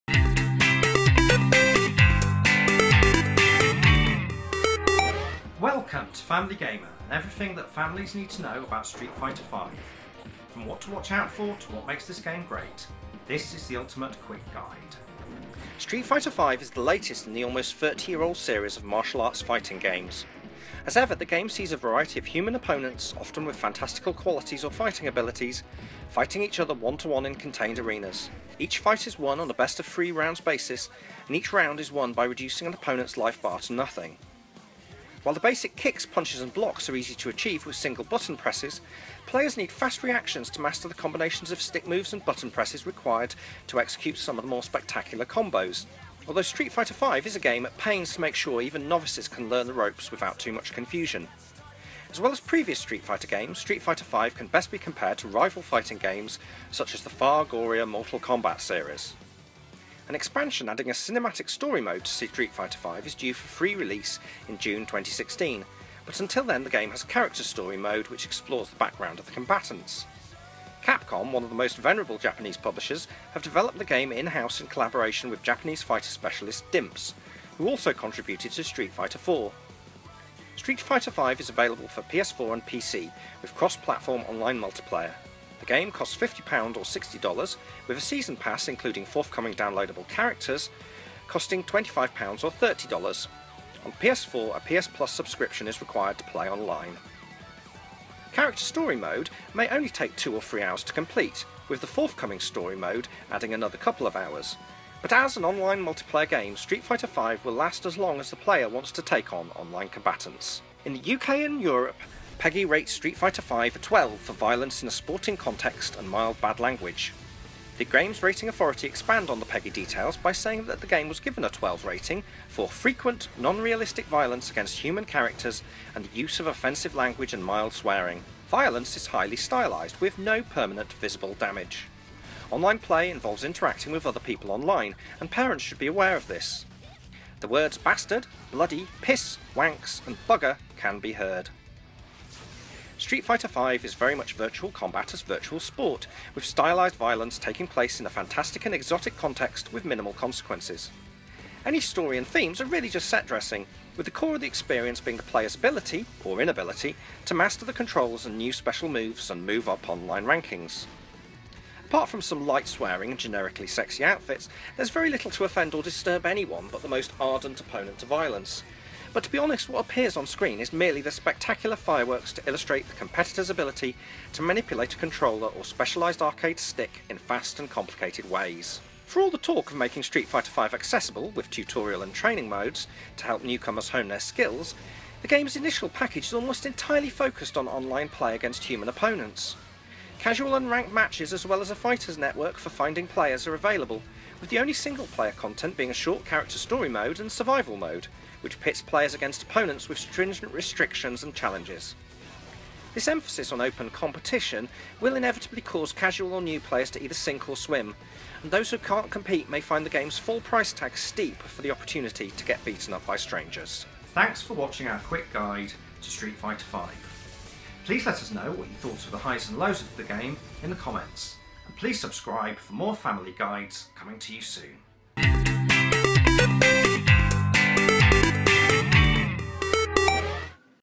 Quick Guide to Street Fighter V (PEGI 12+)
In collaboration with GRA for PEGI and Consumer Information, here's our Quick Guide to Street Fighter V.

1. Genre
Street Fighter V is the latest in the almost thirty year old series of martial arts fighting games. As ever, the game sees a variety of human opponents, often with fantastical qualities or fighting abilities, fighting each other one-to-one in contained arenas. Each fight is won on a best of three rounds basis, and each round is won by reducing an opponent’s life bar to nothing.

While the basic kicks, punches and blocks are easy to achieve with single button presses, players need fast reactions to master the combinations of stick moves and button presses required to execute some of the more spectacular combos, although Street Fighter V is a game at pains to make sure even novices can learn the ropes without too much confusion.

As well as previous Street Fighter games, Street Fighter V can best be compared to rival fighting games such as the far gorier Mortal Kombat series.

2. Story
An expansion adding a cinematic story mode Street Fighter V is due for free release in June 2016, but until then the game has Character Story mode, which explores the background of the combatants.

3. Developer
Capcom, one of the most venerable Japanese publishers, have developed the game in-house in collaboration with Japanese fighter specialists Dimps, who also contributed to Street Fighter IV.

4. Format
Street Fighter V is available for PS4 and PC, with cross-platform online multiplayer. The game costs £50 or $60. A season pass including forthcoming downloadable characters costs £25 or $30. On PS4 a PS Plus subscription is required to play online.

5. Duration and Difficulty
Character Story Mode may only take 2 or 3 hours to complete, with the Story Mode due later in the year adding another couple of hours, but as an online multiplayer game Street Fighter V will last as long as the player wants to take on online combatants.

6. PEGI Rating
In the UK and Europe, PEGI rates Street Fighter V a 12 for violence in a sporting context and mild bad language.

7. GRA Additional Cunsumer Information
The GRA expand on their PEGI details by saying that the game was given a 12 rating for ‘frequent, non-realistic violence against human characters and the use of offensive language and mild swearing.’ Violence is ‘highly stylised’ with ‘no permanent visible damage’. Online play involves interacting with other people online, and parents should be aware of this. The words ‘bastard, bloody, piss, wanks and bugger’ can be heard.

8. Themes

Street Fighter V is very much virtual combat as virtual sport, with stylised violence taking place in a fantastic and exotic context with minimal consequences. Any story and themes are really just set dressing, with the core of the experience being the player’s ability - or inability - to master the controls and new special moves and move up online rankings.

Apart from some light swearing and generically sexy outfits there’s very little to offend or disturb anyone but the most ardent opponent to violence, but to be honest what appears on screen is merely the spectacular fireworks to illustrate the competitors ability to manipulate a controller or specialised arcade stick in fast and complicated ways.

9. Why people play:

For all the talk of making Street Fighter V accessible, with Tutorial and Training modes, to help newcomers hone their skills, the game’s initial package is almost entirely focused on online play against human opponents. Casual and Ranked matches as well as a Fighters Network for finding players are available, with the only single player content being a short Character Story Mode and Survival Mode, which pits players against opponents with stringent restrictions and challenges.

This emphasis on open competition will in a way that will inevitably cause casual or new players to either sink or swim, and those who can’t compete may find the game’s full price tag steep for the opportunity to get beaten up by strangers.